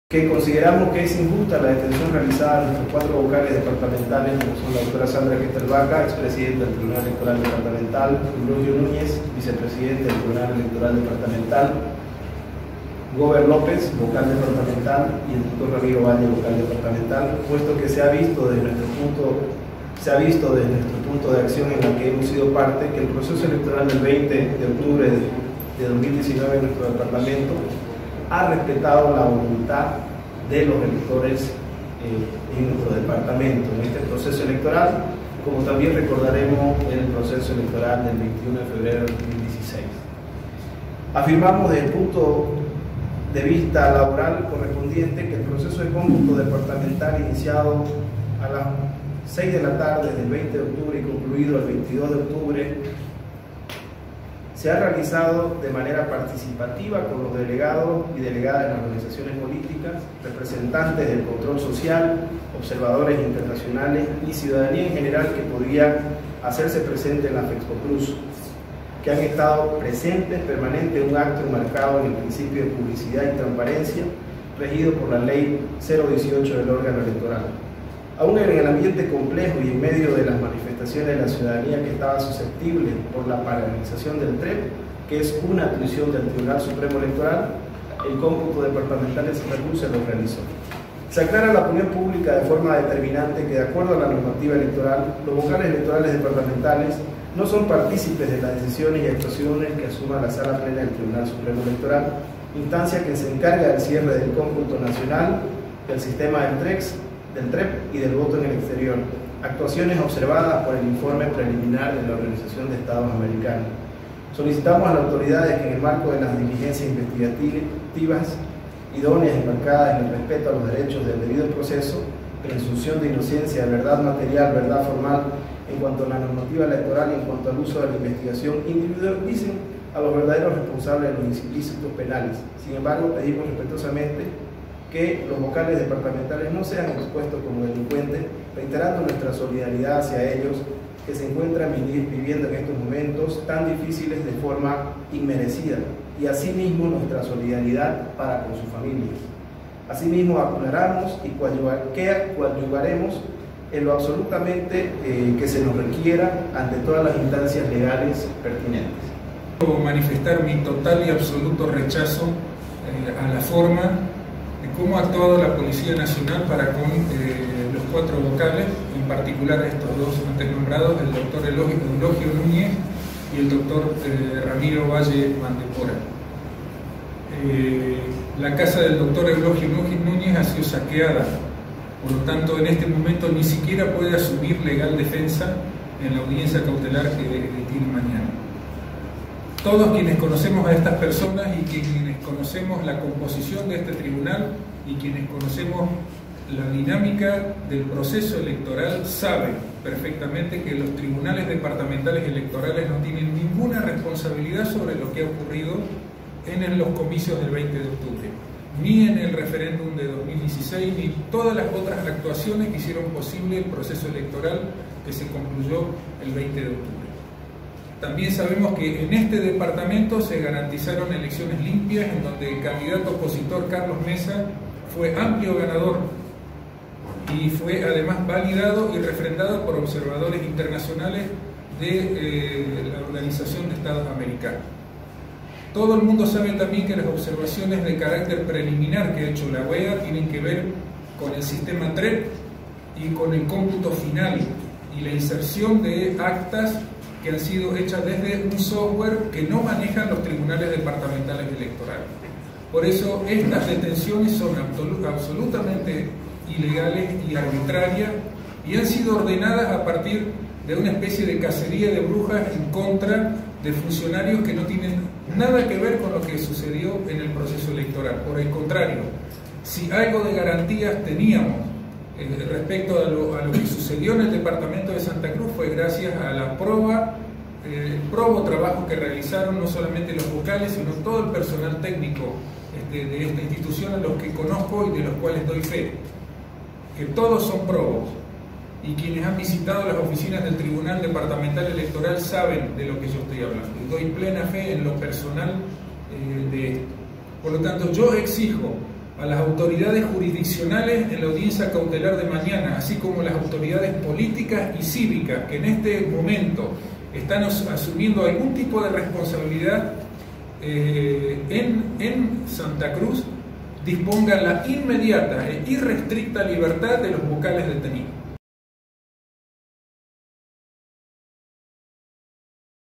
Trabajadores del TED de santa Cruz y Plataforma aseguran trabajo transparente de Vocales detenidos y
Trabajadores del TED de santa Cruz y Plataforma aseguran trabajo transparente de Vocales detenidos y exigen liberación inmediata